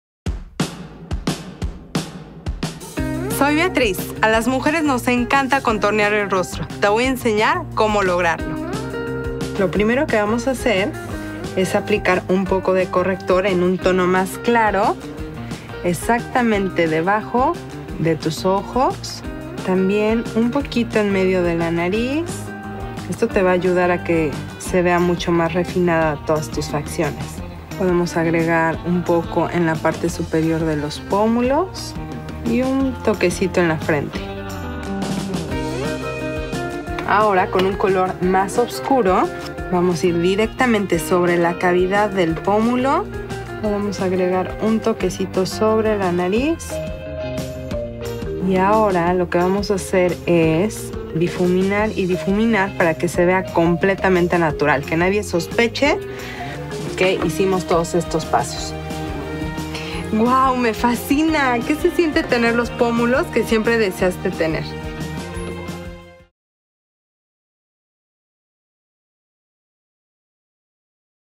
맥 Instant Artistry -- 컨투어링의 모든 것
맥 수석 아티스트 Beatriz Cisnero가 입체적인 얼굴을 표현할 수 있는 컨투어링 팁을 공개합니다. 지금 바로 만나보세요!